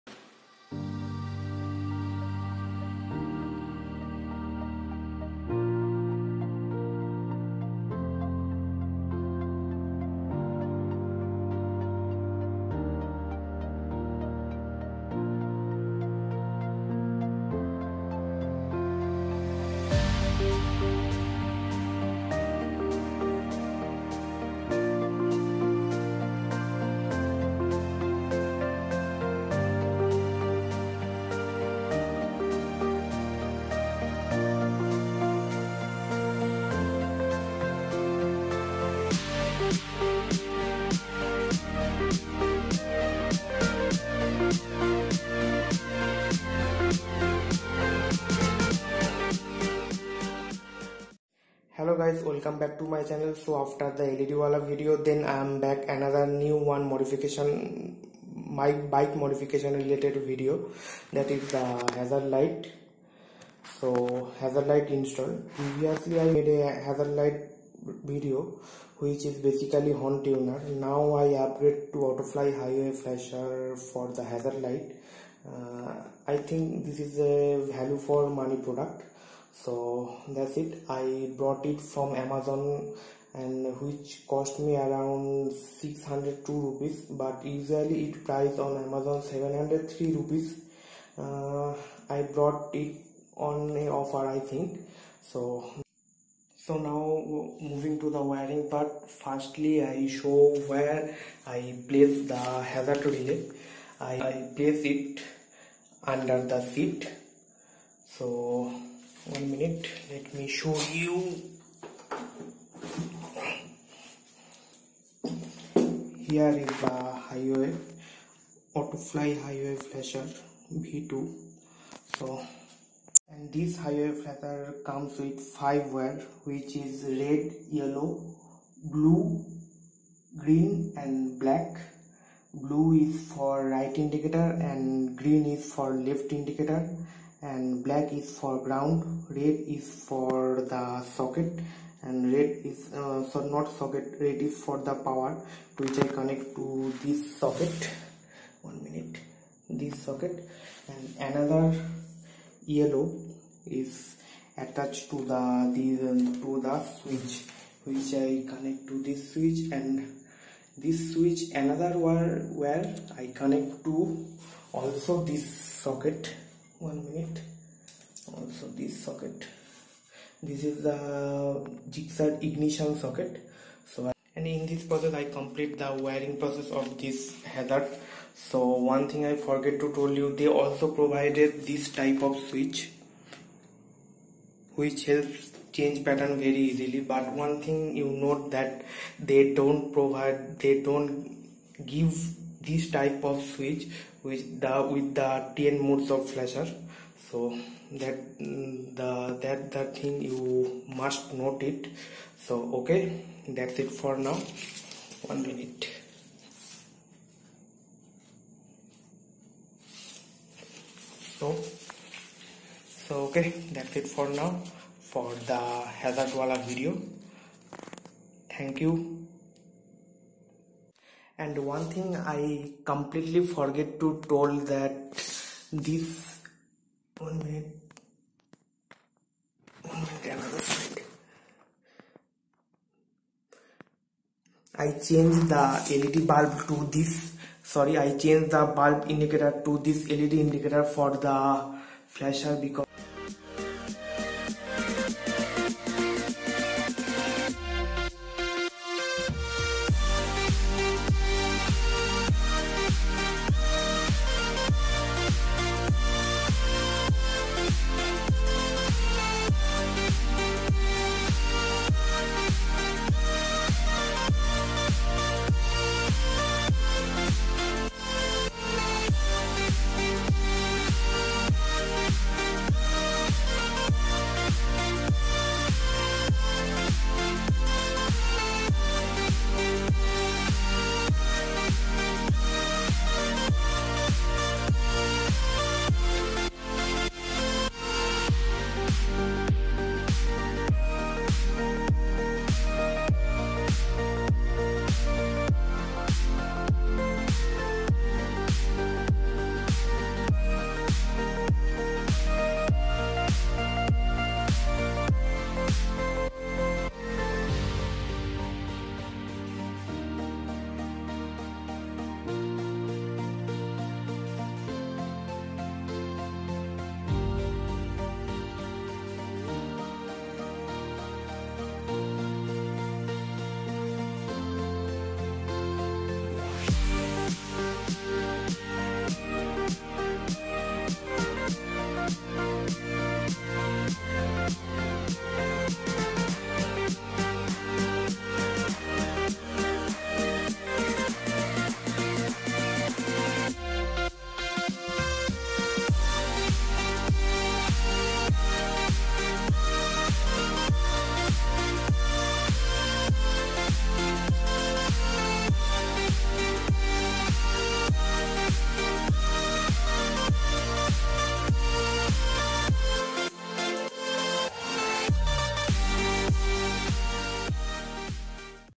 Installing Hazard Light In Suzuki Gixxer Sf With 30 Patterns ||[ER]
HAZARD LIGHT DONE WITH AUTOFY HIGHWAY INDICATOR FLASHER  IN SUZUKI GIXXER SF
Installation & wearing video.
Brought It From Amazon India...
Link :
Do like
[ER]